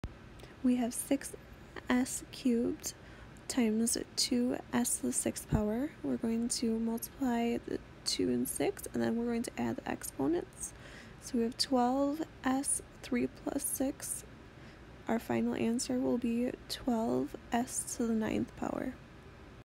Exponents: Simplify 6s^3 * 2s^6
Tiala from SVSU Micro Math helps you simplify an expression involving exponents using properties of exponents.

Problem: Simplify 6s^3 * 2s^6